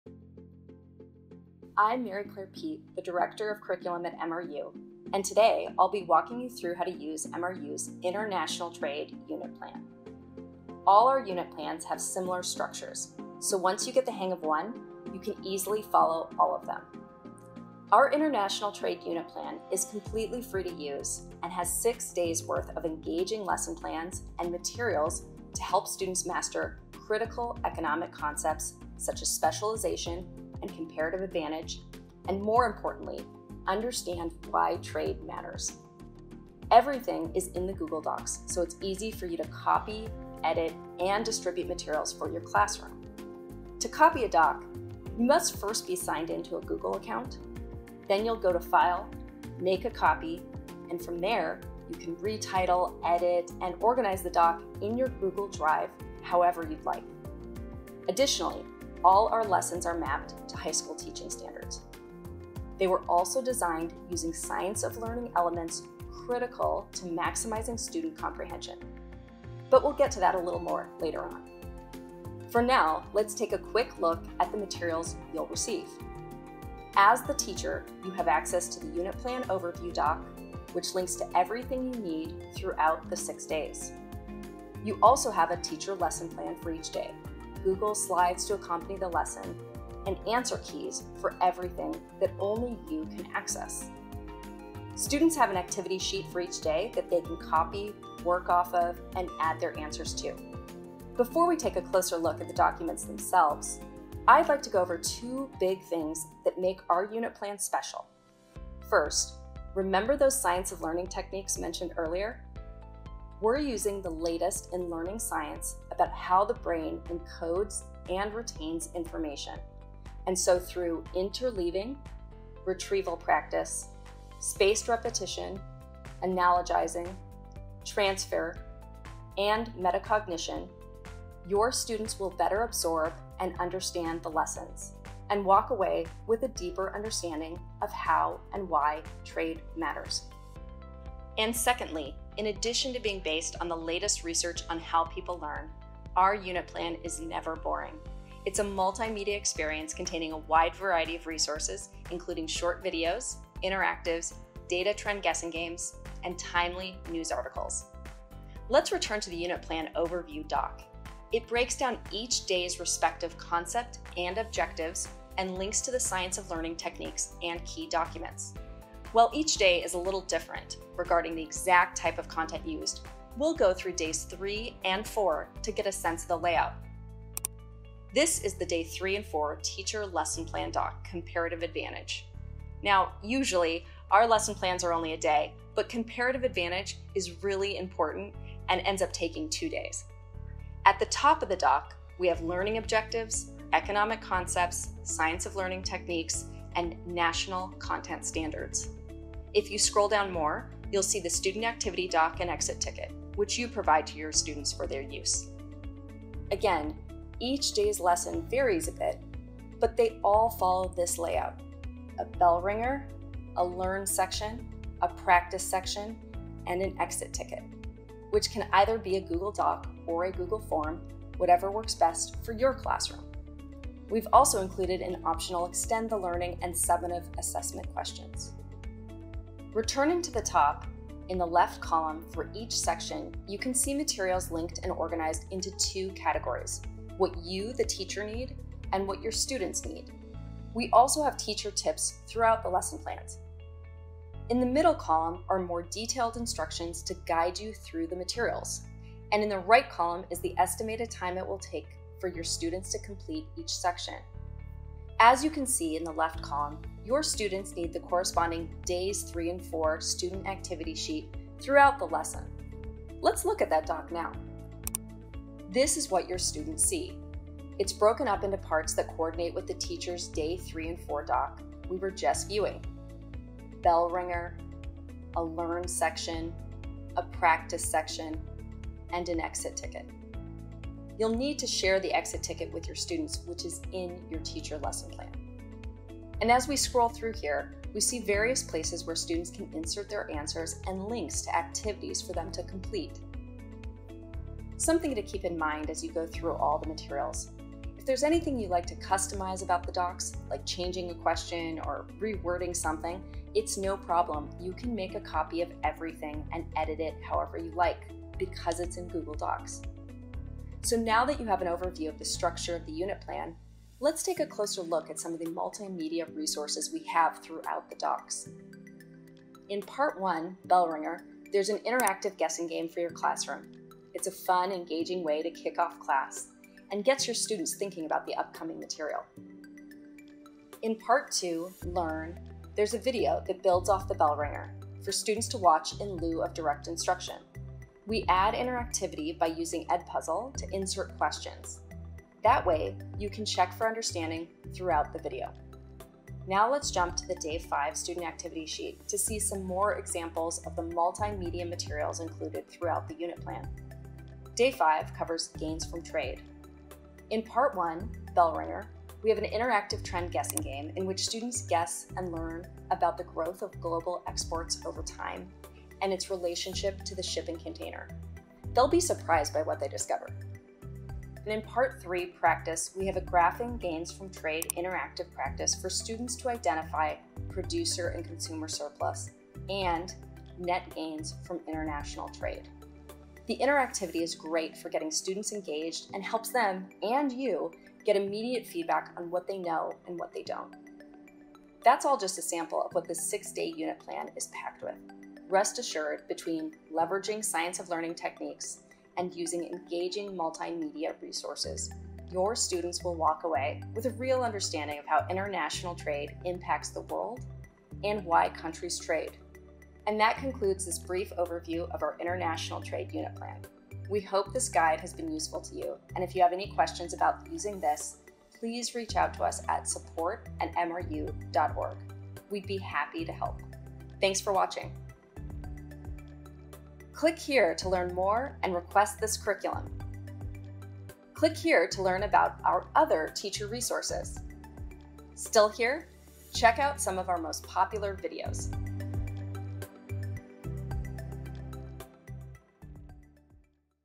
Teaching the International Trade Unit Plan
Our free, 6 day International Trade Unit Plan is sure to help students master critical economic concepts such as specialization and comparative advantage and, more importantly, understand why trade matters.